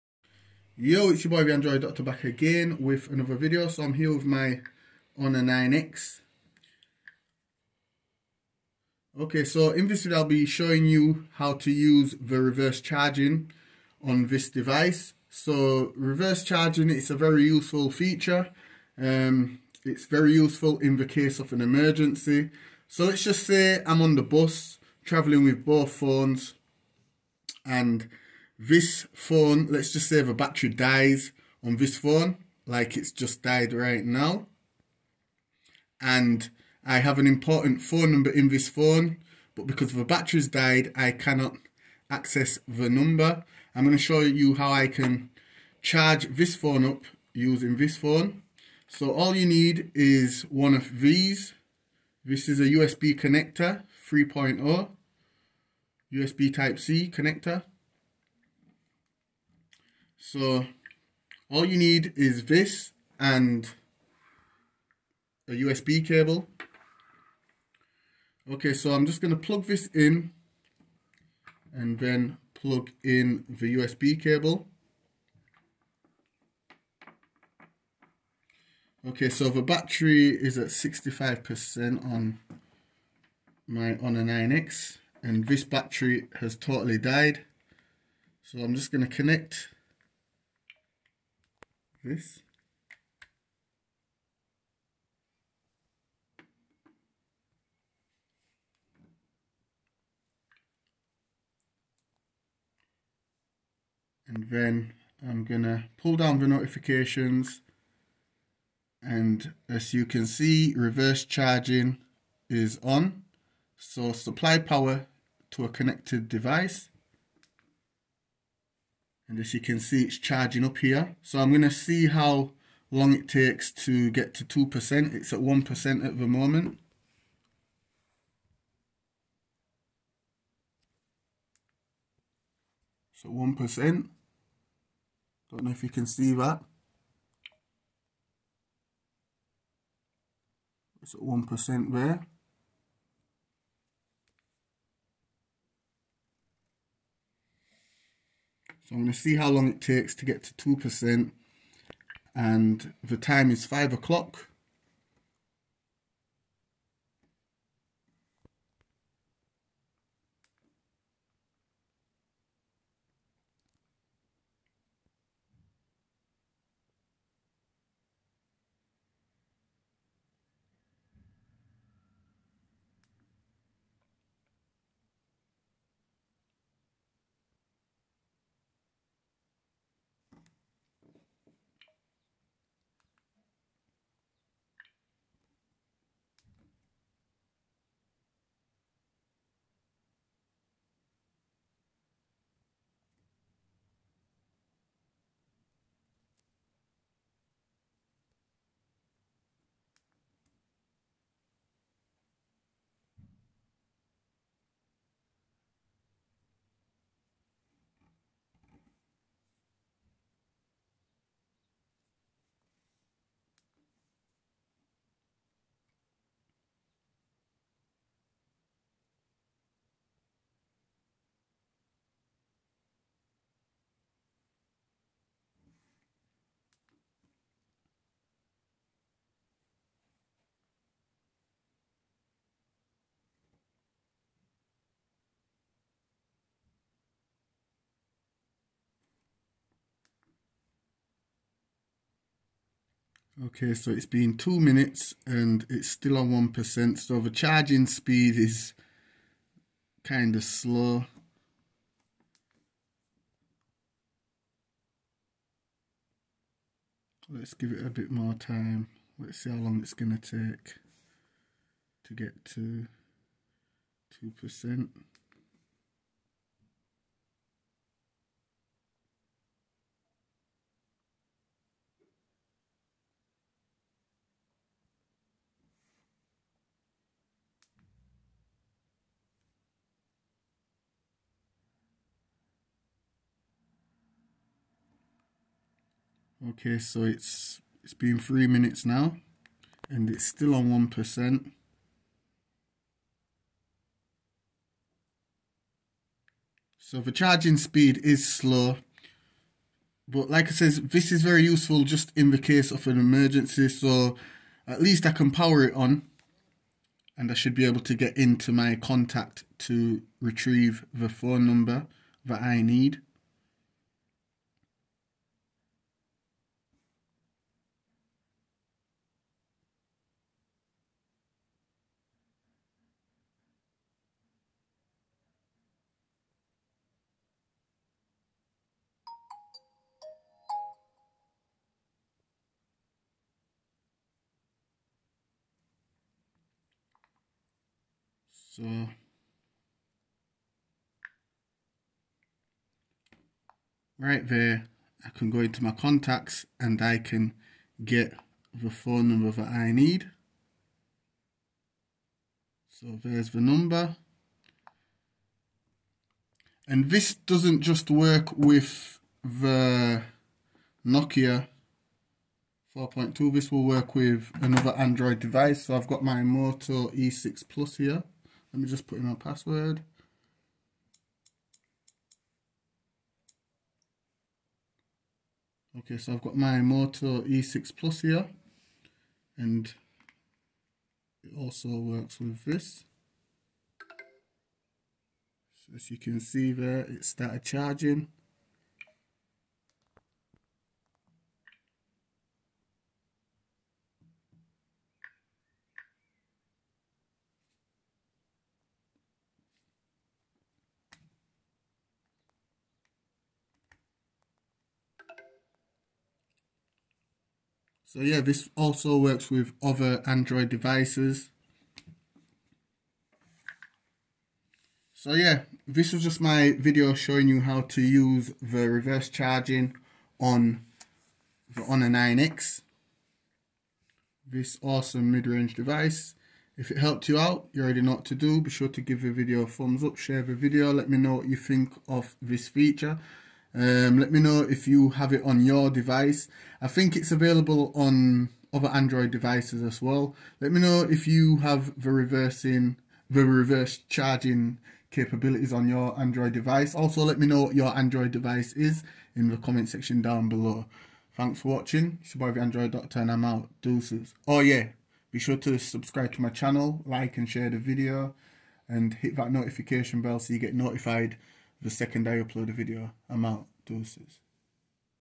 Honor 9X Reverse Charging| Emergency Charging On The Go!!
in this video i show how to use reverse charging on the Honor 9X